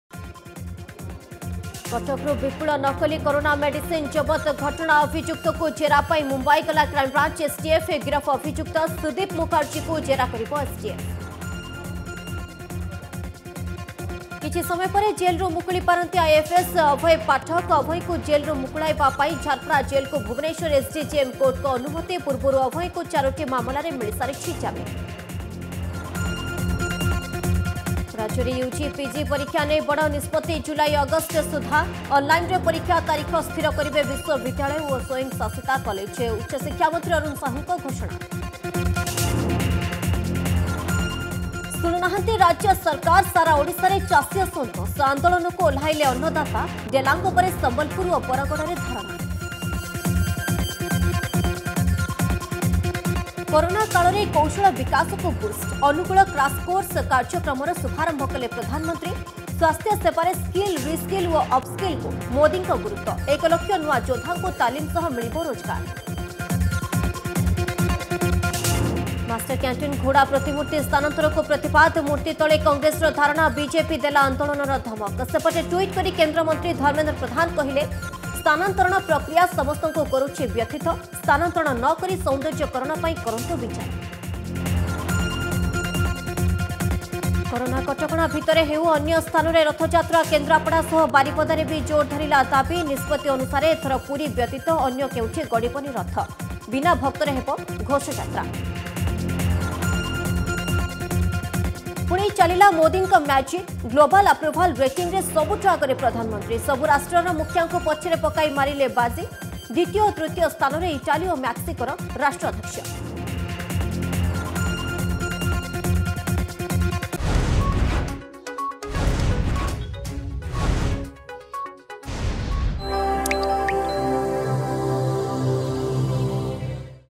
1806 4PM HL FB G
Headlines 4PM
+ କଟକରୁ ବିପୁଳ ନକଲି କୋରୋନା ମେଡିସିନ ଜବତ ଘଟଣା
+ କିଛି ସମୟ ପରେ ଜେଲରୁ ମୁକୁଳିପାରନ୍ତି IFS ଅଭୟ ପାଠକ
+ ରାଜ୍ୟରେ  UG, PG ପରୀକ୍ଷା ନେଇ ବଡ଼ ନିଷ୍ପତ୍ତି
+ ଶୁଣୁନାହାନ୍ତି ରାଜ୍ୟ ସରକାର
+ କରୋନା କାଳରେ କୌଶଳ ବିକାଶକୁ ବୁଷ୍ଟ୍
+ ମାଷ୍ଟରକ୍ୟାଣ୍ଟିନ ଘୋଡ଼ା ପ୍ରତିମୂର୍ତ୍ତି ସ୍ଥାନାନ୍ତରକୁ ପ୍ରତିବାଦ